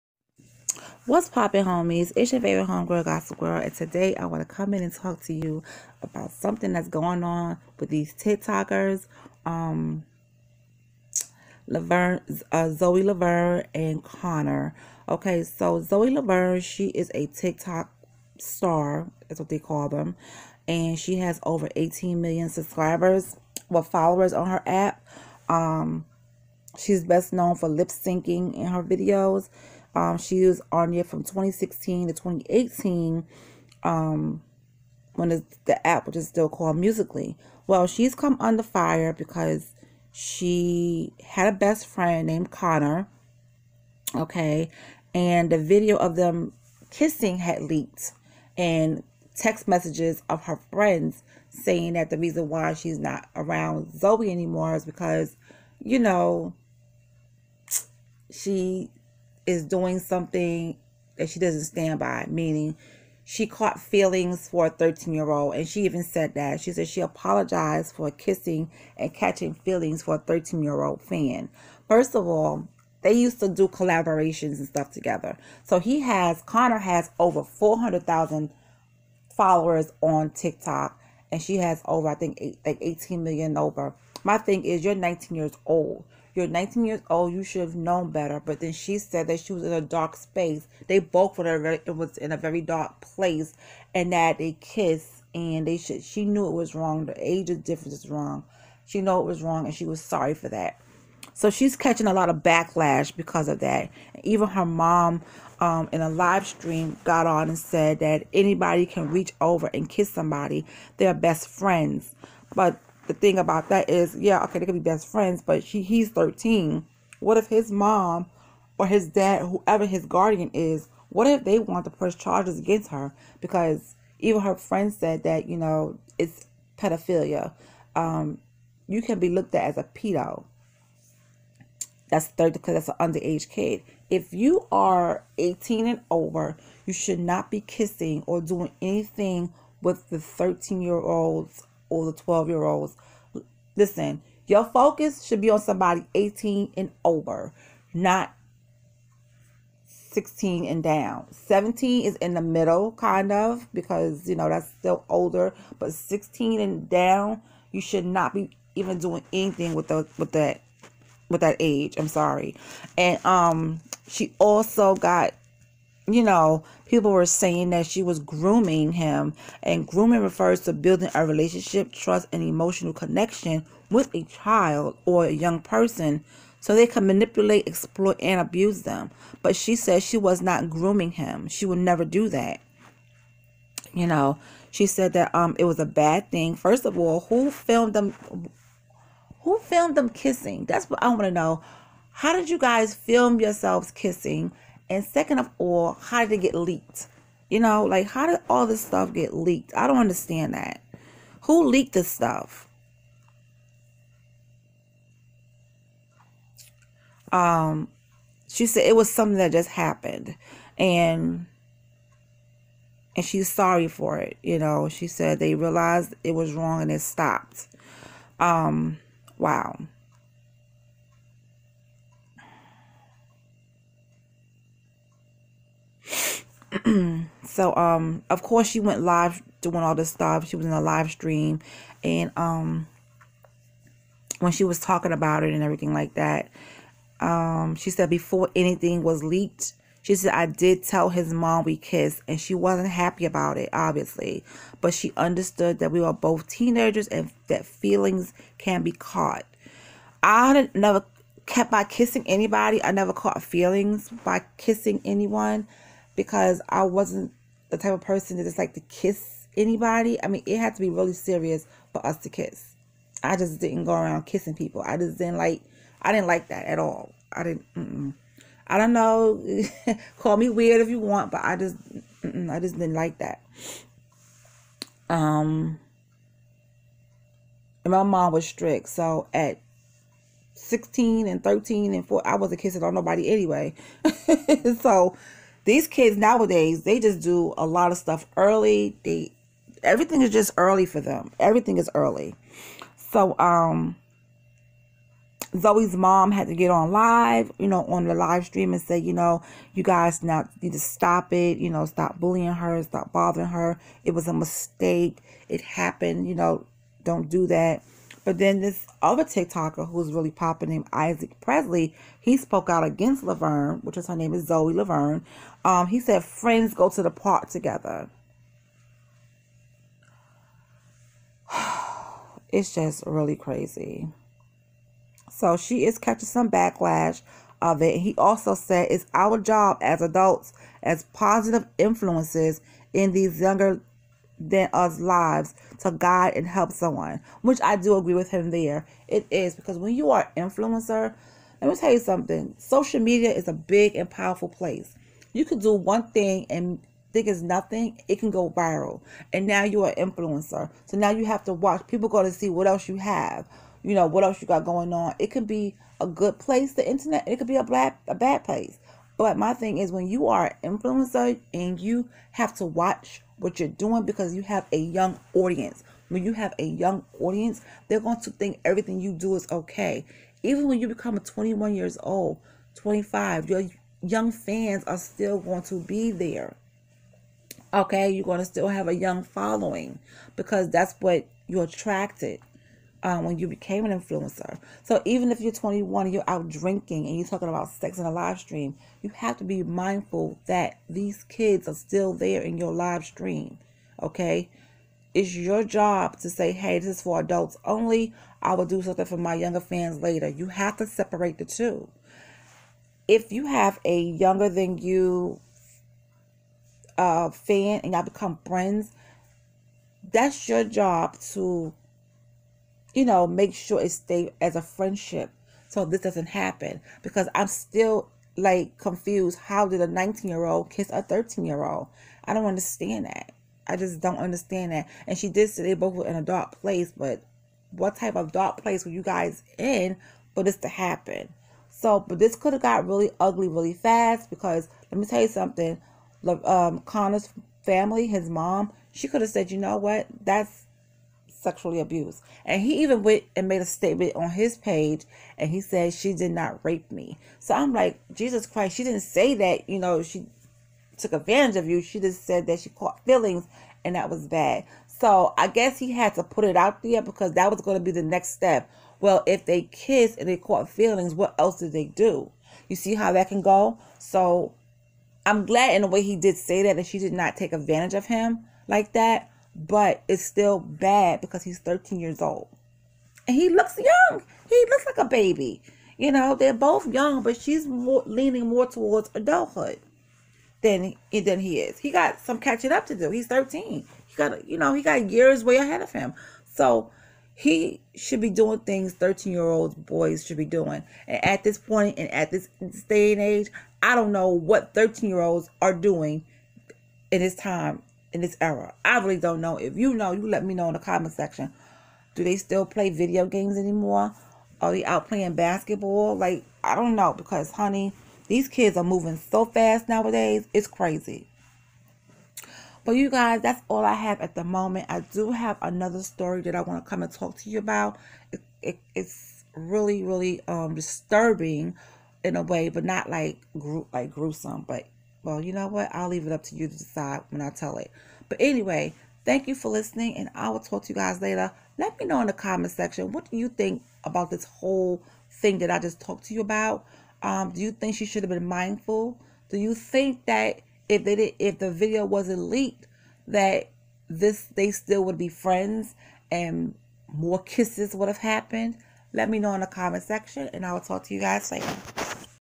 Tik Tok Famous Zoe Lavern 19 Catches Heat For Her Behavior With Connor Who's 13 Years Old!
Zoe Laverne catches backlash for her behavior with Connor.

These kids are a mess!